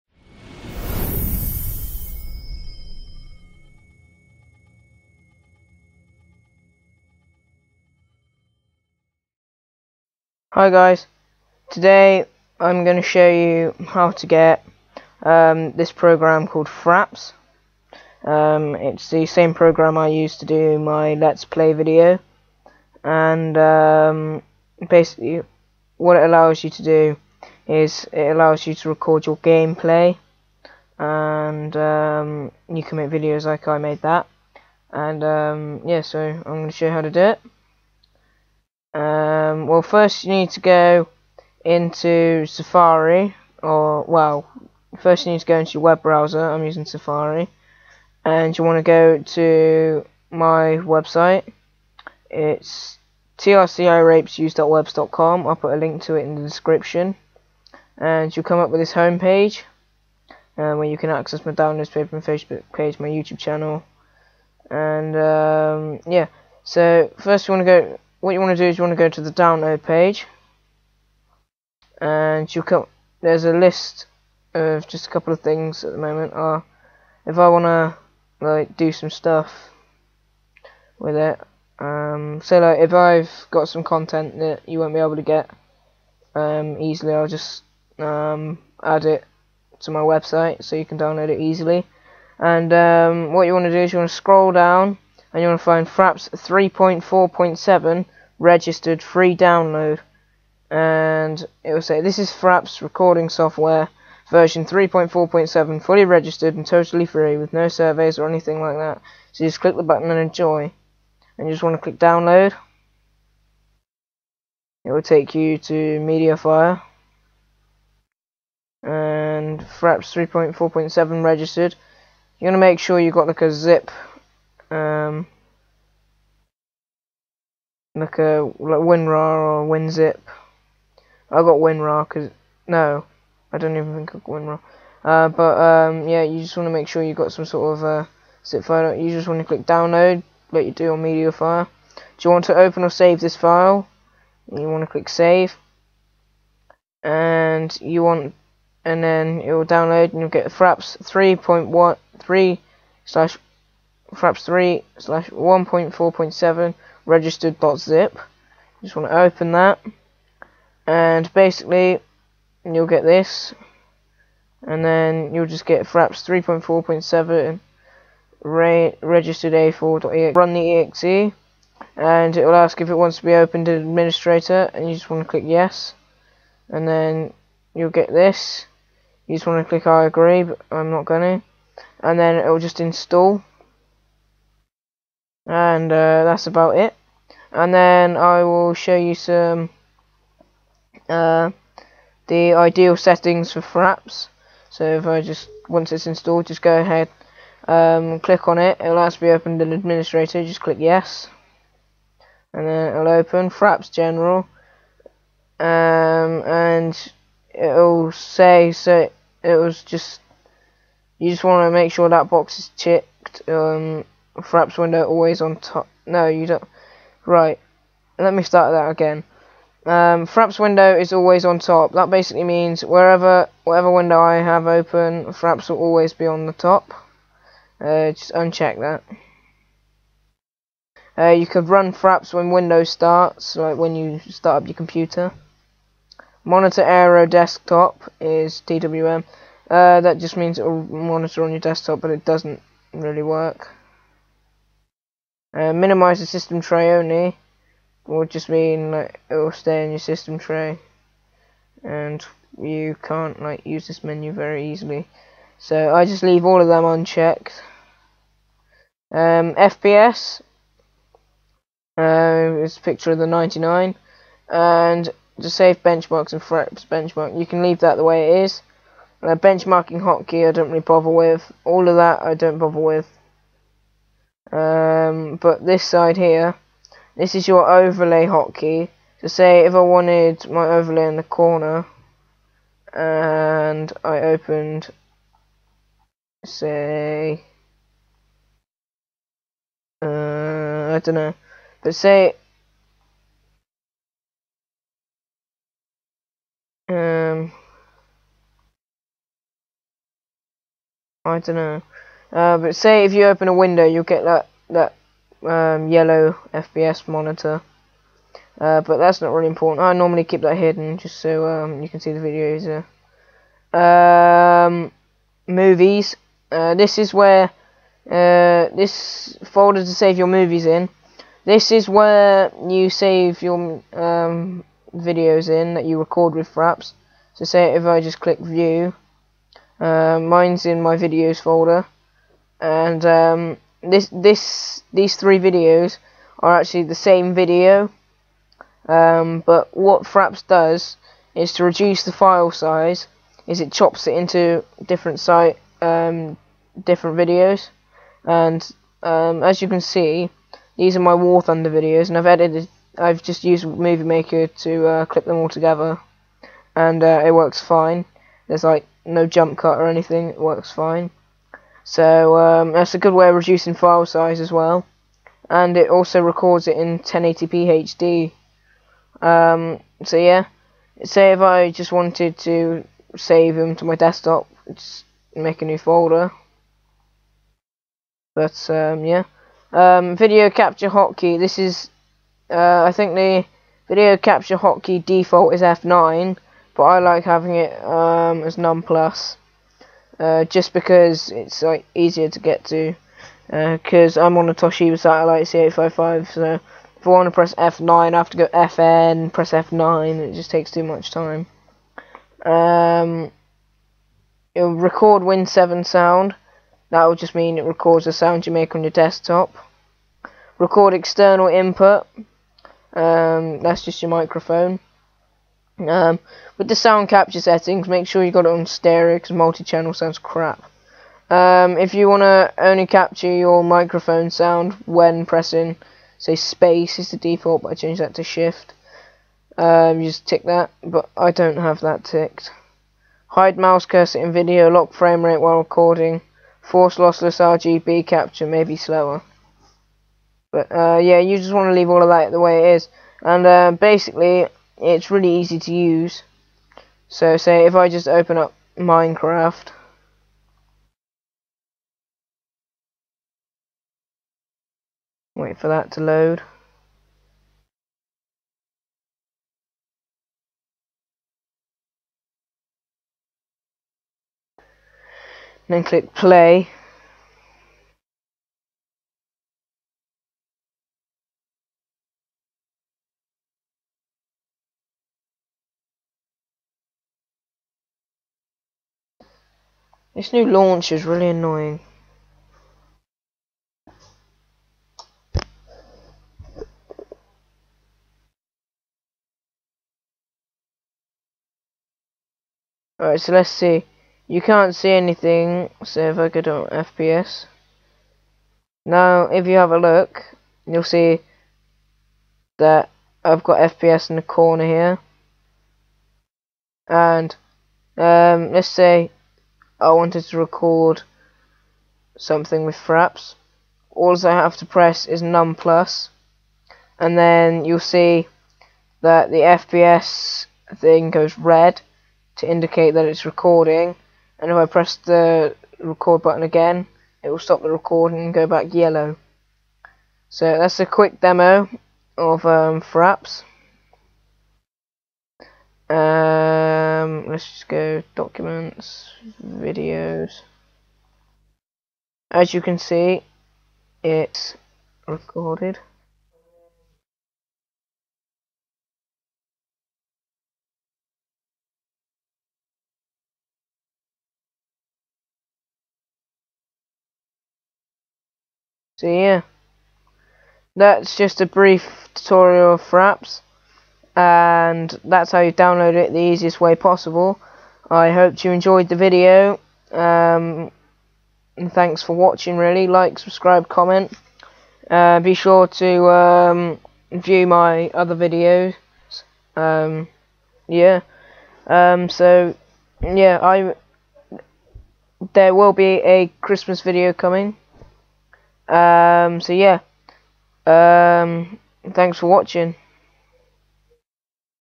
How To Get FRAPS For Free Easily
Hey guys hope you enjoy this video i think it's an easy way of recoding your gameplay.